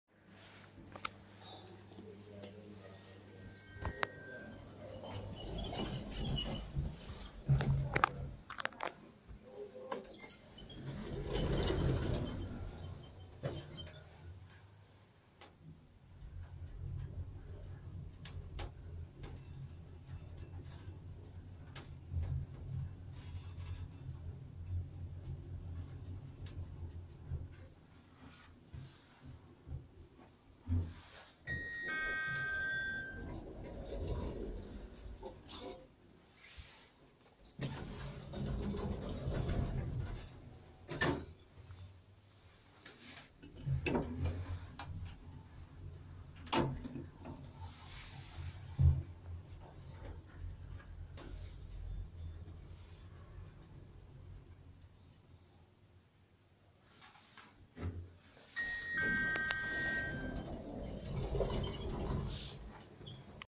Richie Hart Lifts- Westport Coast Hotel, Westport
Here are two nice and rare Richie Hart lifts at the Westport Coast Hotel in Westport Quay on the outskirts of Westport, Co.Mayo.

Make- Richie Hart
Year- 1989*
Motor- Traction
Floors-
  0- Main Floor
   1- Bedrooms
   2- Bedrooms
   3- Bedrooms
   4- Bedrooms

*The year is according to a few hotel websites that says the Year of Construction of the hotel is 1989.